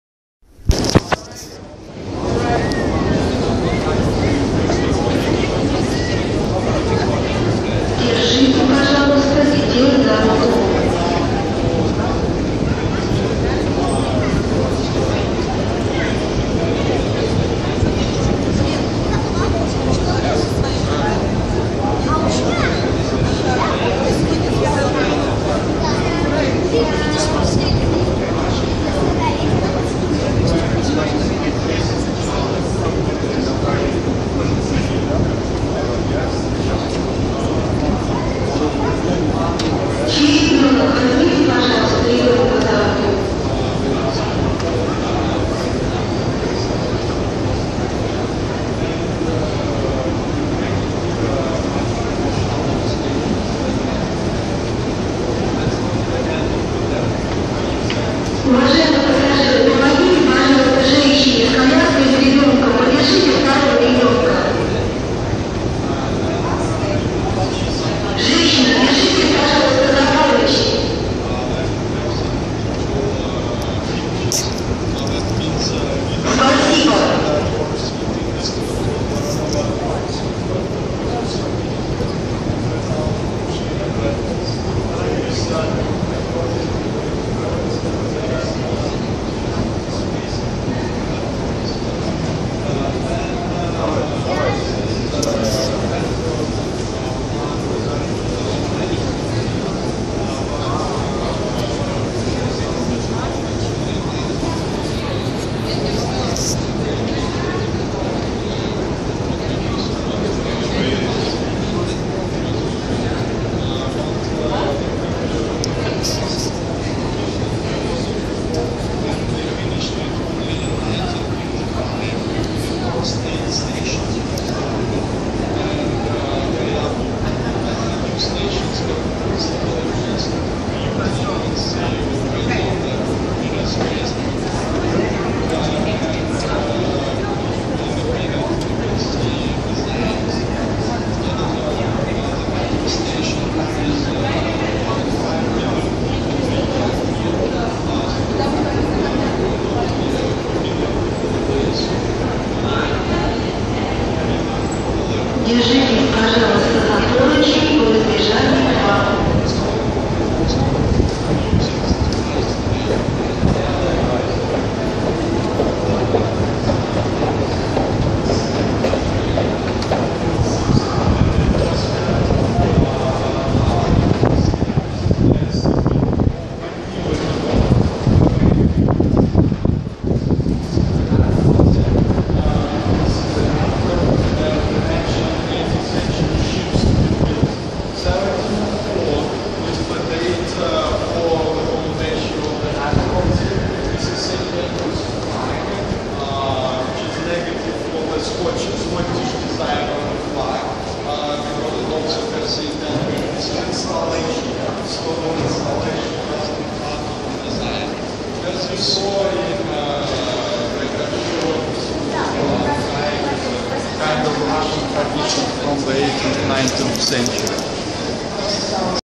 St. Petersburg, Russia subway; escalator is the longest in the world--approx. 5 minutes ride.
Saturday, May 7, 2016. Videographed by Barbara Burke and J. M. B.